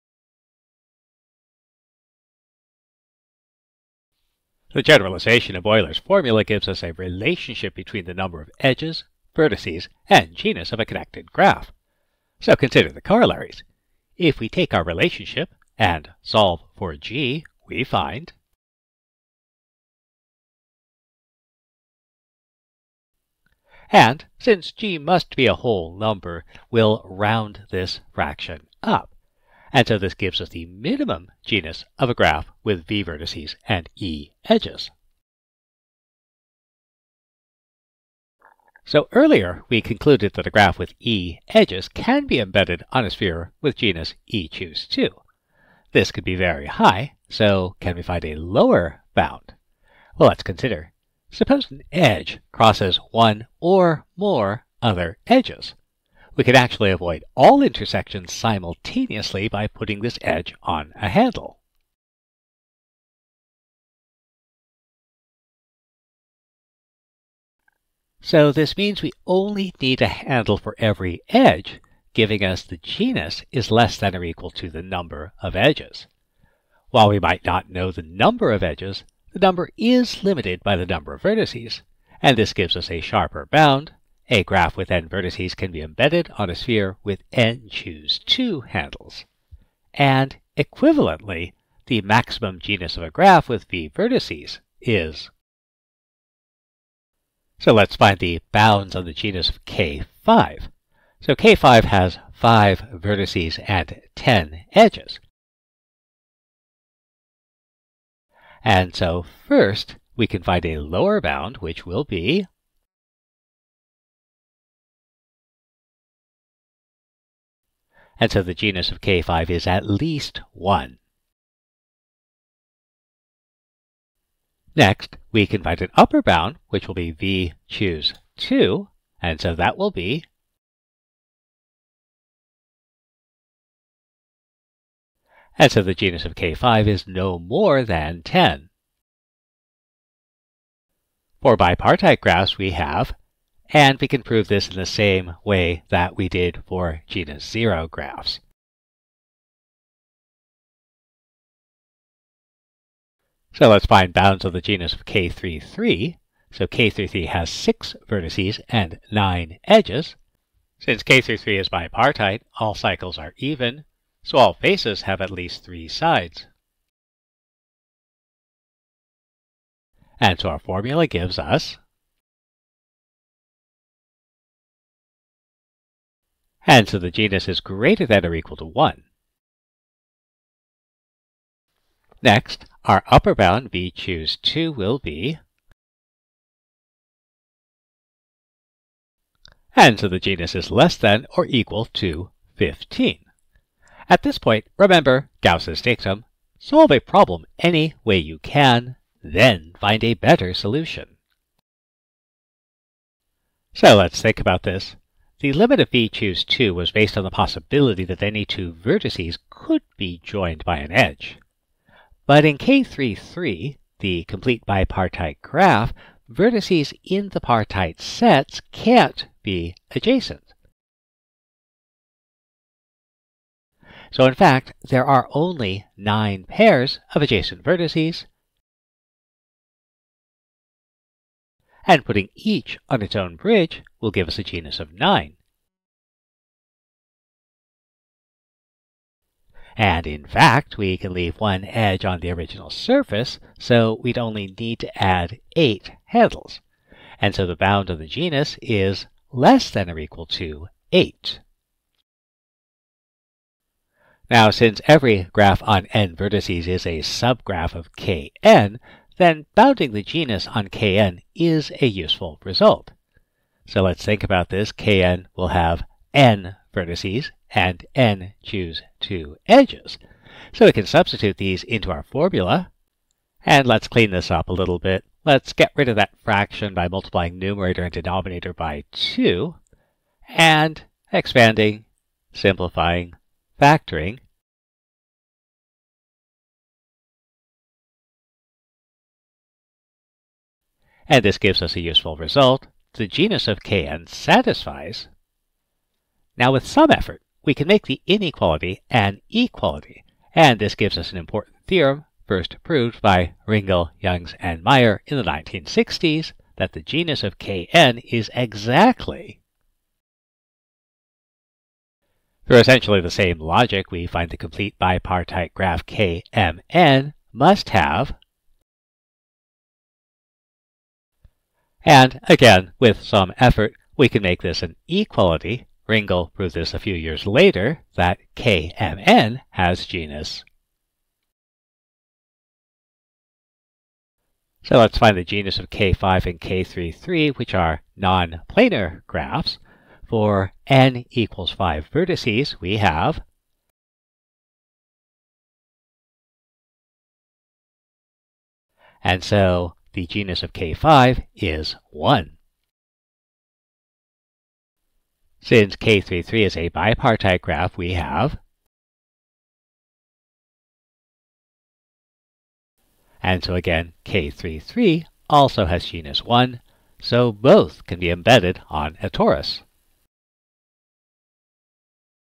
More Graphs, More Surfaces
Finding the genus of a graph.